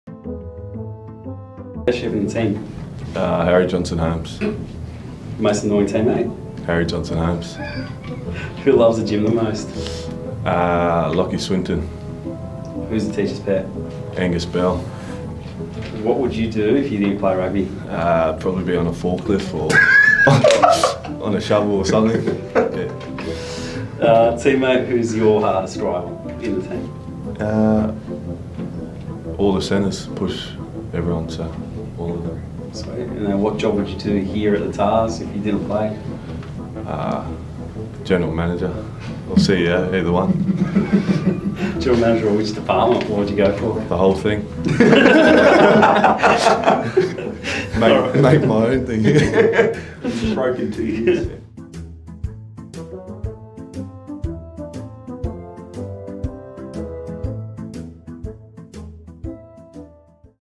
Tahs Rapid Fire - Lalakai Foketi
Rapid FIRE 🔥 with Tahs star Centre Lalakai Foketi!

Next Home Match ➡️ vs. The Rebels

📆 Saturday 13 May
⏰ 7.35pm Kickoff
📍Allianz Stadium